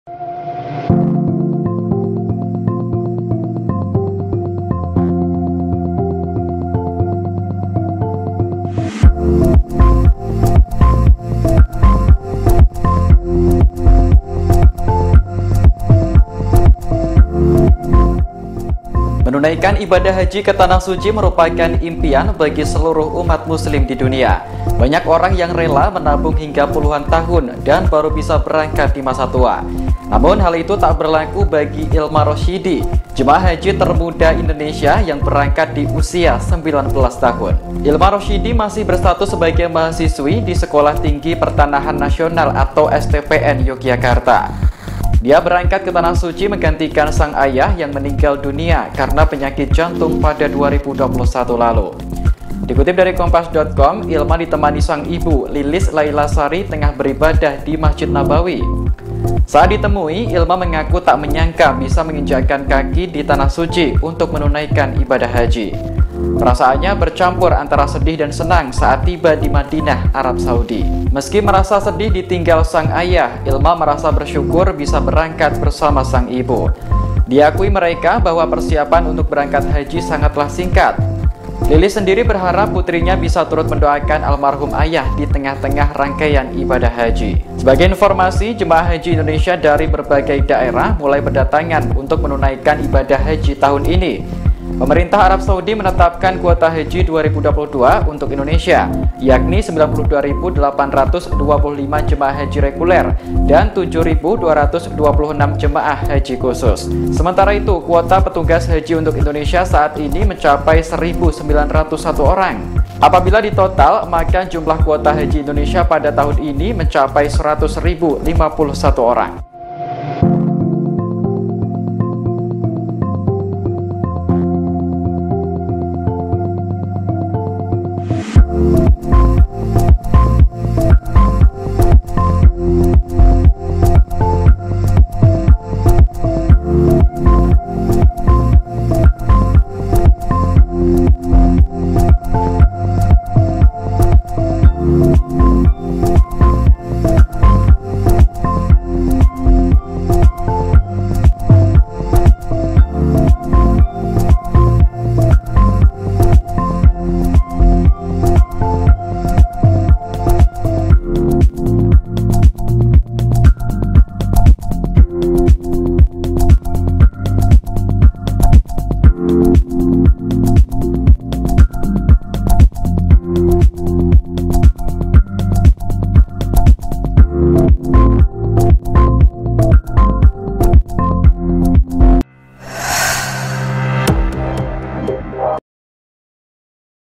Jemaah Haji Termuda dari Indonesia 2022 yang Berangkat di Usia 19 Tahun, Ini Dia Sosok Ilma Rosyidi
Menunaikan ibadah Haji ke Tanah Suci merupakan impian bagi seluruh umat Muslim di dunia.

Banyak orang yang rela menabung hingga puluhan tahun dan baru bisa berangkat di masa tua.

Namun, hal itu tak berlaku bagi Ilma Rosyidi, jemaah Haji termuda dari Indonesia yang berangkat di usia 19 tahun.

Ilma Rosyidi masih berstatus sebagai mahasiswi di Sekolah Tinggi Pertanahan Nasional (STPN) Yogyakarta.